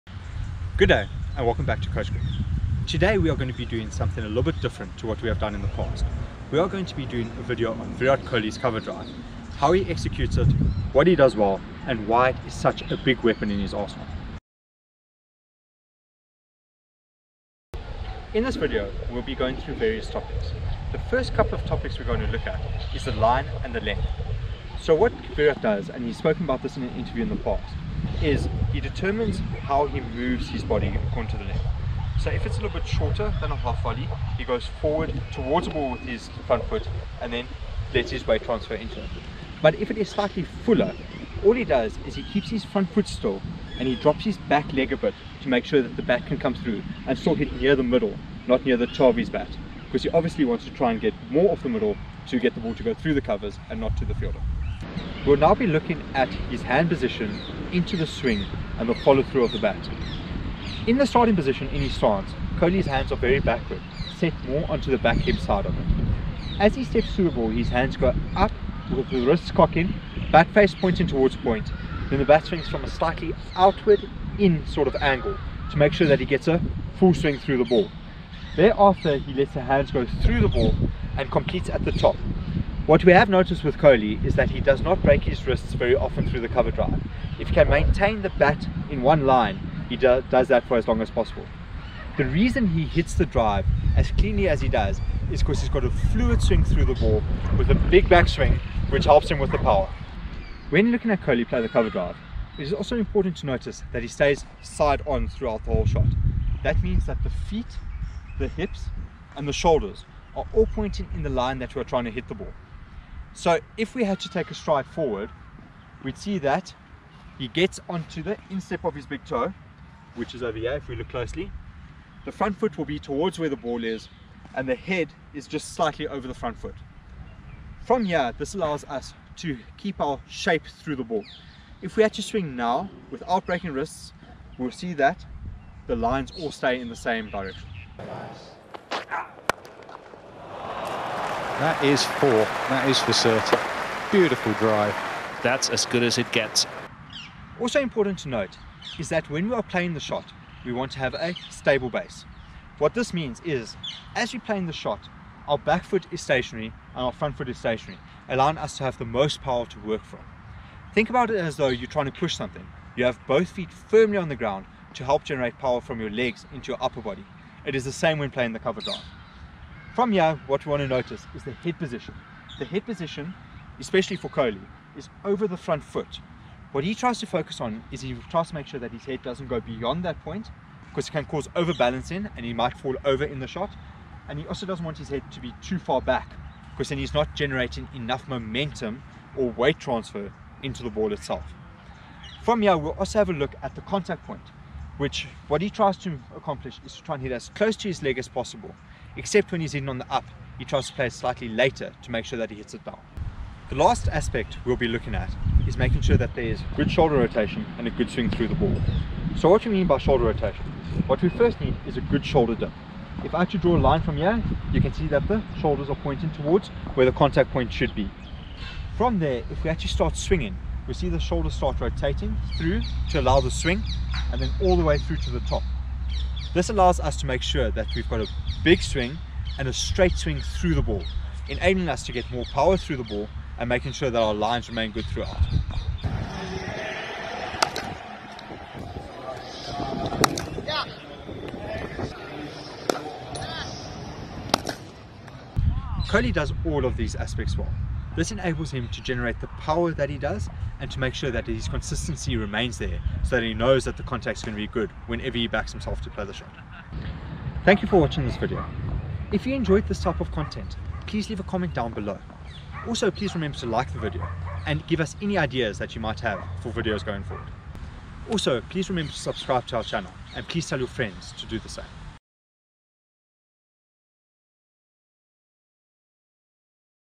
How to cover drive like VIRAT KOHLI | Virat Kohli Batting Technique
In this video we cover one of the greatest batsmen to ever play the game. Virat Kohli has one of the best cover drives out there, and in this video we cover Virat Kohli's batting technique, what he does well and why his cover drive it is such a massive weapon in his arsenal.

This is the complete guide - How to cover drive like Virat Kohli.

How to bat like Virat Kohli